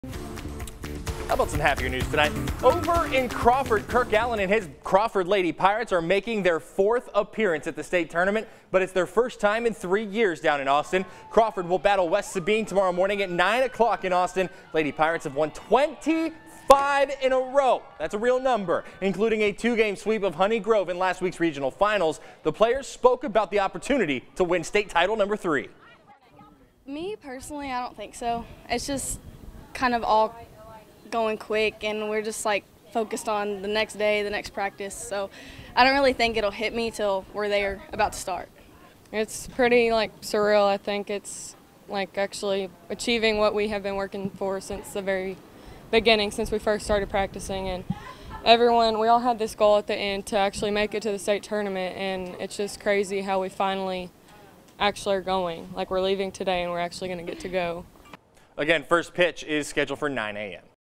Crawford softball seeks third state title
Crawford softball is just two wins away from snagging its third state title. The Lady Pirates will battle West Sabine 9 a.m. Wednesday in Austin.